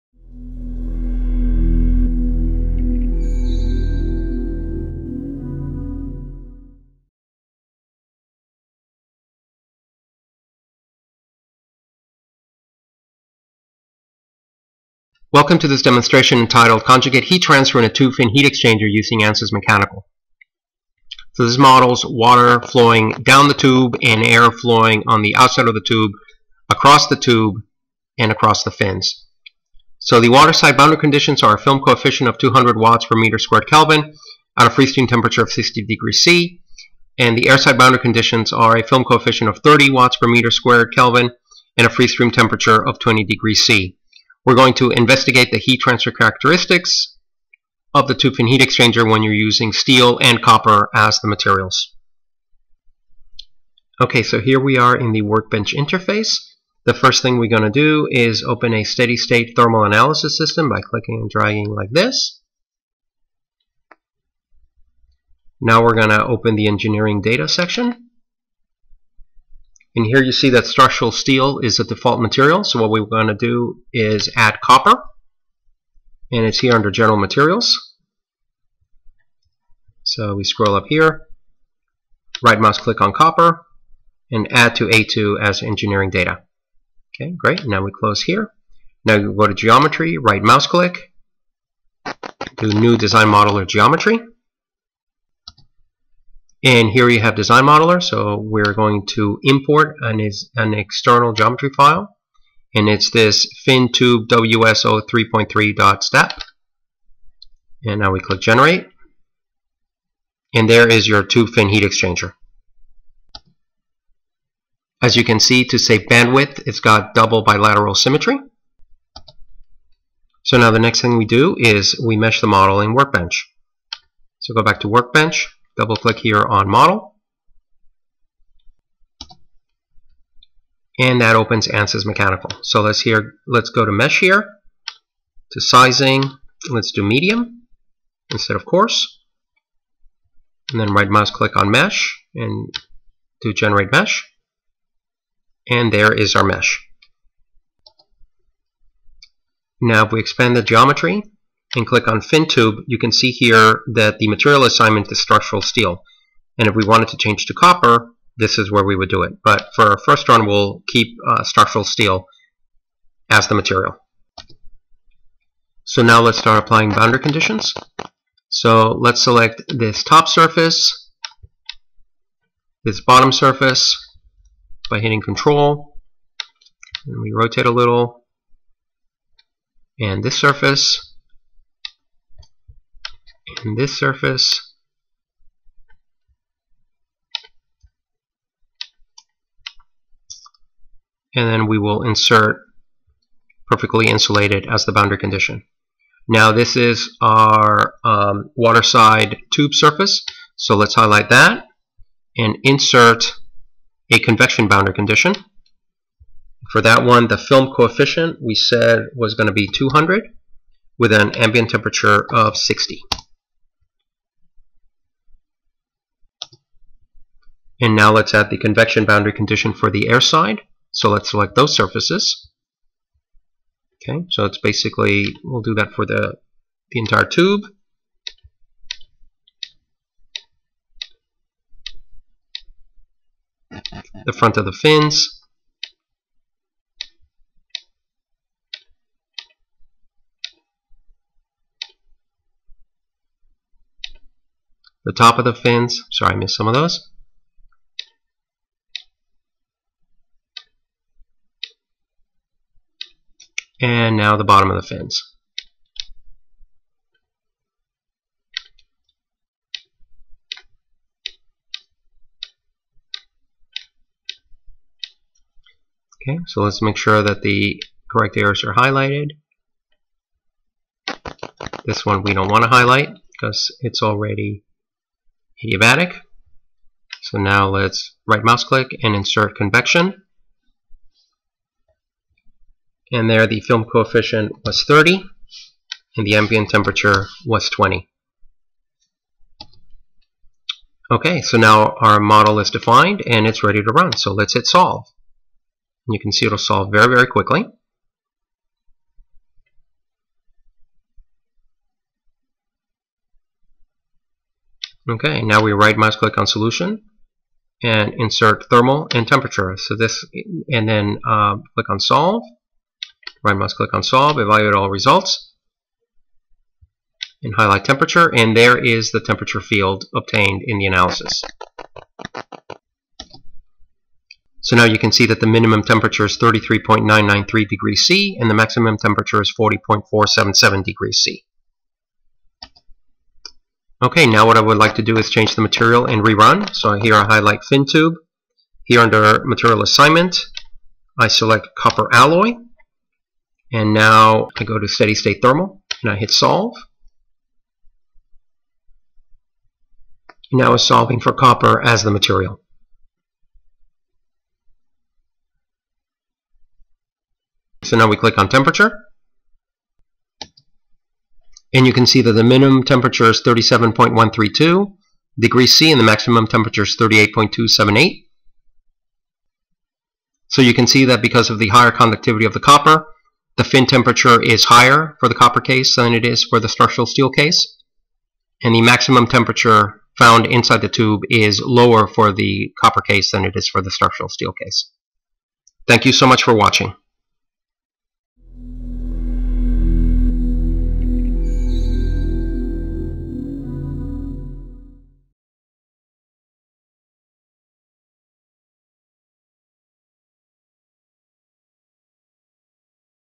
ANSYS Student: Conjugate Heat Transfer in a Tube Fin Heat Exchanger Using ANSYS Mechanical
This video shows how to model conjugate heat transfer in a tube fin heat exchanger using ANSYS Fluent.